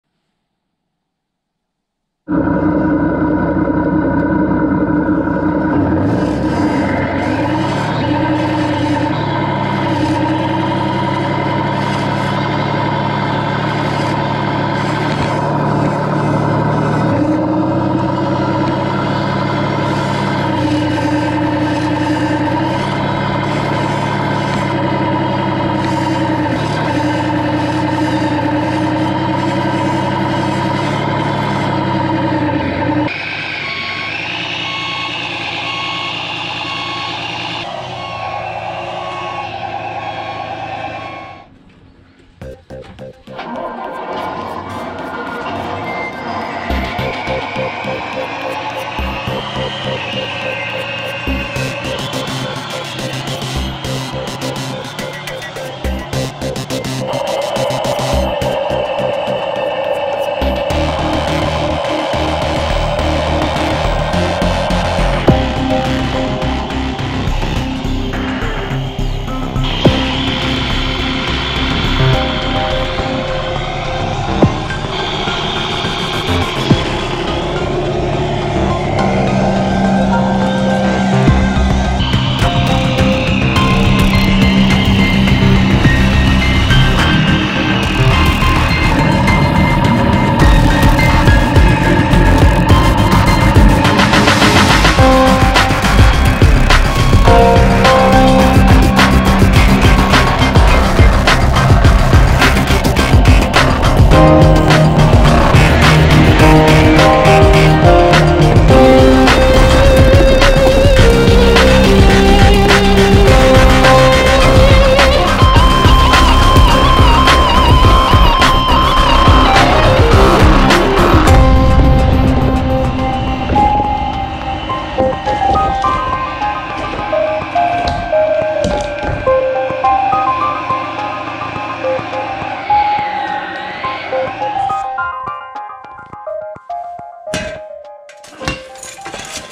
RC Truck hauling firewood / RC Holzlieferung
Hauling firewood with my 1:8 scale articulated dump truck.
Brennholzlieferung mit meinem ferngesteuerten 1:8 Knicklenker.